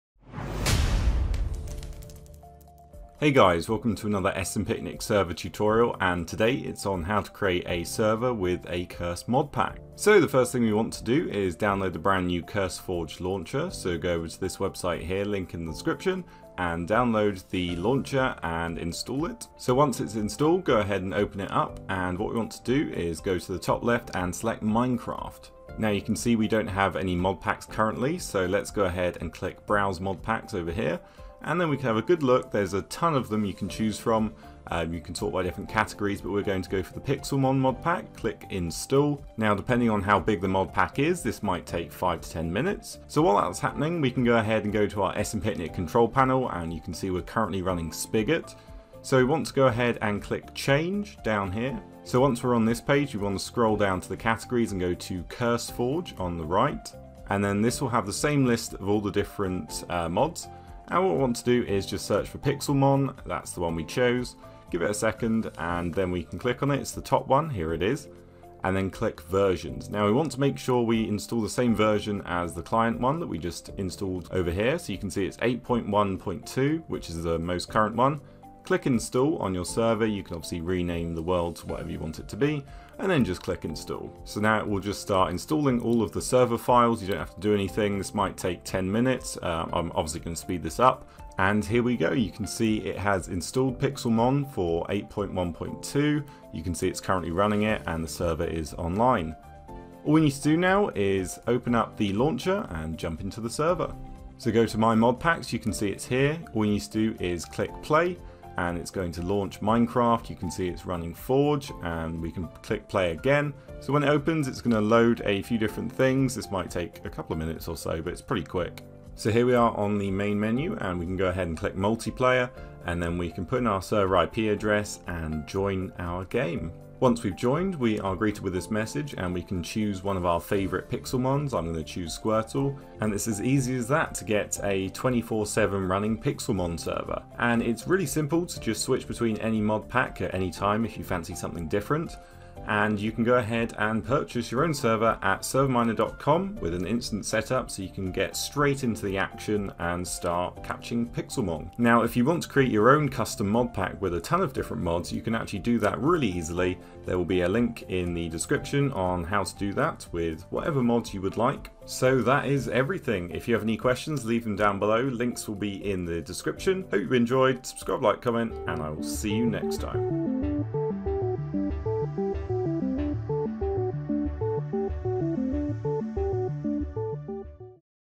How to create a Modpack Server using ANY CurseForge Modpack (Overwolf) [PAID]
With ServerMiner, we offer one click-installation so that you can instantly create your own modded Minecraft server with any available modpack on Overwolf's CurseForge app. Instantly purchase your Minecraft server with instant delivery

1) The first step is to open the ServerMiner control panel and navigate to the Installer and click the "CurseForge" modpack category.
2) Next step is to search for the CurseForge modpack which you'd like to play and then click the "Versions" button.
3) You can then click the "Install" button next to the version you'd like to play on.
4) Once the modpack has installed, you can load up the modpack on your computer through the CurseForge app and then connect to your server.